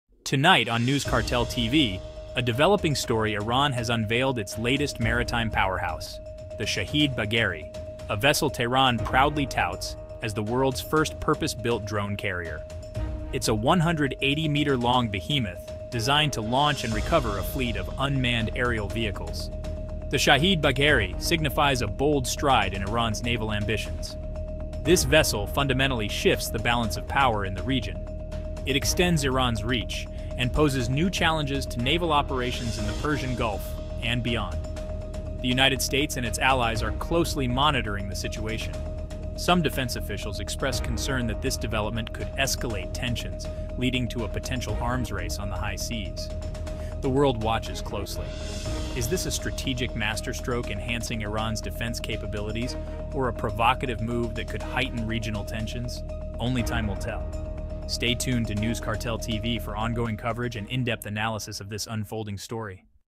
Iran's New Drone Carrier: Game Changer Or Provocation?
Iran has unveiled the Shahid Bagheri, the world's first purpose-built drone carrier, revolutionizing naval warfare with advanced UAV launching and recovery capabilities. This strategic vessel significantly enhances Iran's naval strength, potentially shifting the balance of power in the Persian Gulf and raising serious concerns among the U.S. and its allies. U.S. defense officials are closely monitoring the situation, cautioning about possible escalations and an emerging arms race. Dive deep into the geopolitical implications and technical marvels of this groundbreaking development!